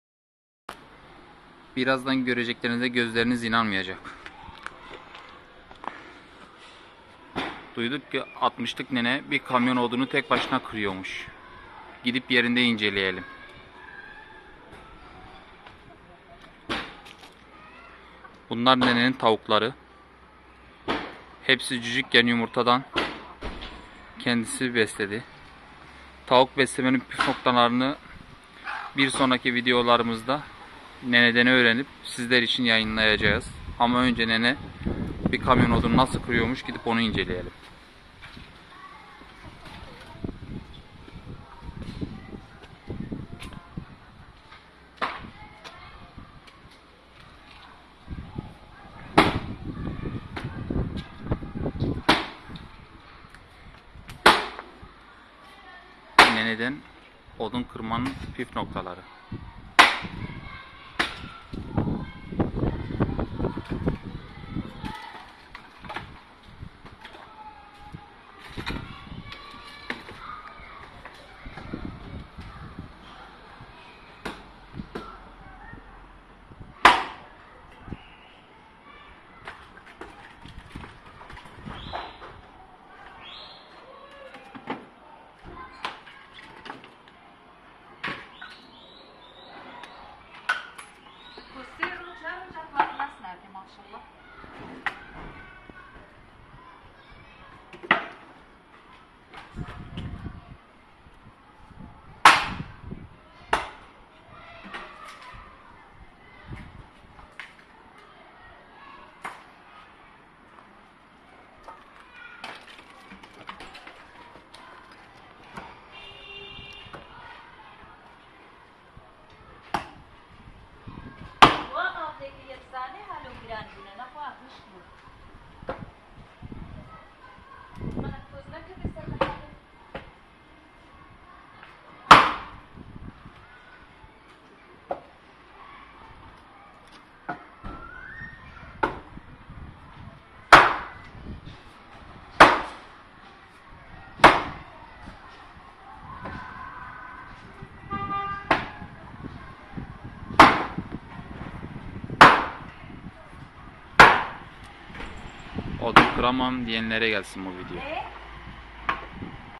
60'lık Nene Bir Kamyon Odunu Kırıyor.(HD ÇEKİM)
GÖZLERİNİZE İNANAMAYACAKSINIZ.60 lık nene bir kamyon odunu baltayla kırıyor.HAS anadolu kadını daha varmış.Buyrun değişik bir video izleyin.çok ilginç.